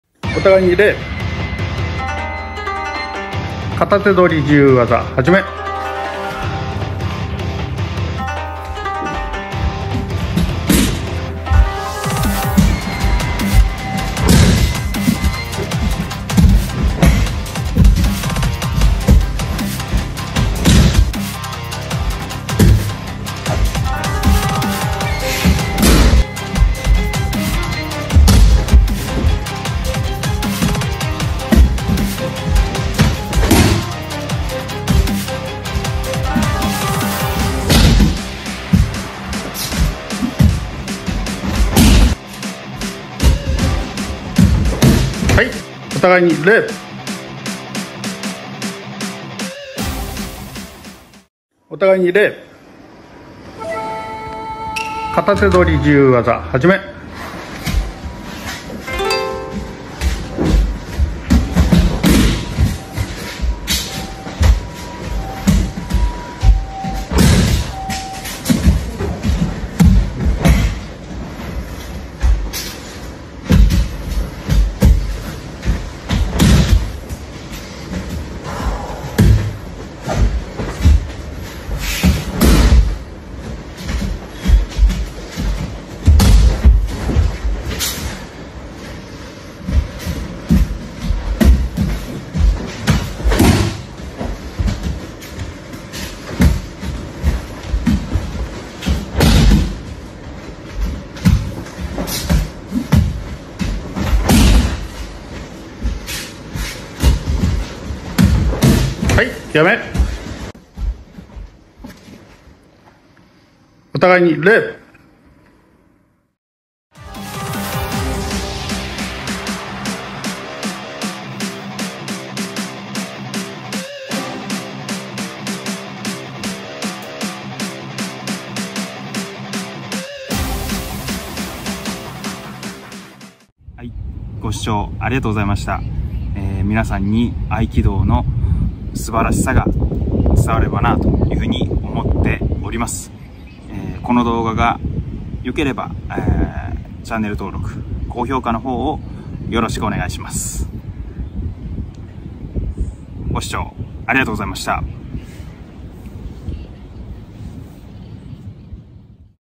あらい道場〜新春片手取り【直井 初段】〜
春風の

行きし日思う

あらい道場

【直井 初段】による新春片手取り自由技をご覧下さい。